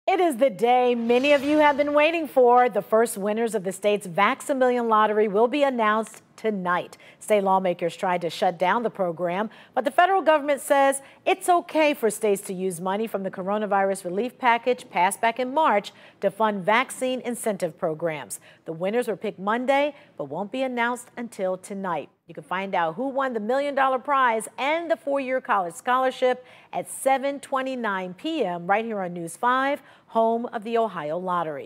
How to watch the Vax-a-Million $1 million drawing live Wednesday night
The result of the first of five Vax-a-Million drawings will be broadcast live on Wednesday at 7:29 p.m. You can watch the result and find out who the first $1 million winner is live on News 5 Cleveland, the official partner station of the Ohio Lottery.